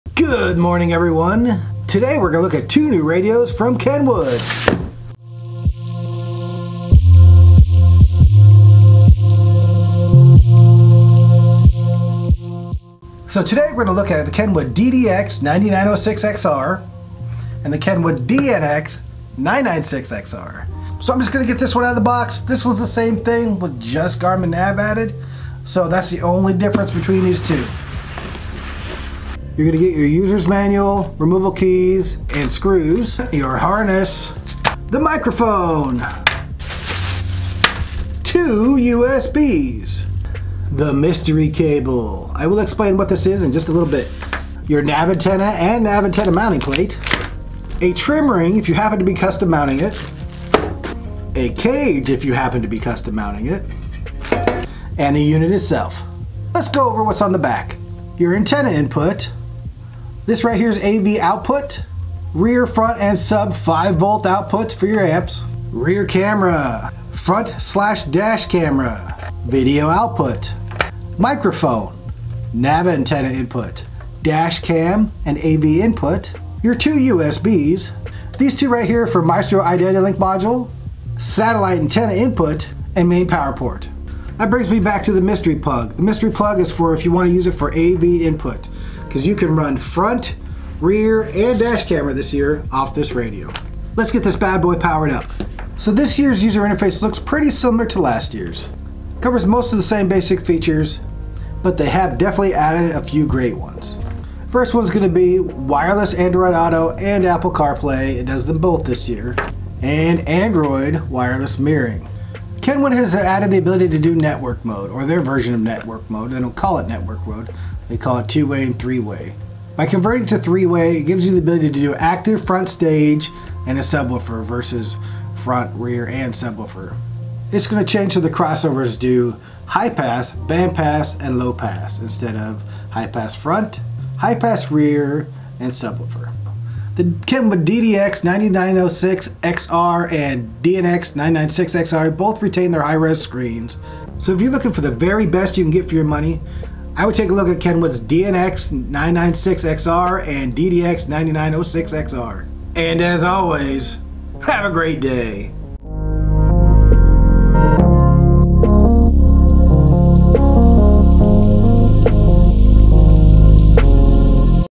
Kenwood DNX996XR & DDX9906XR
A quick look at whats new om Kenwoods new flagship indash multimedia receivers!1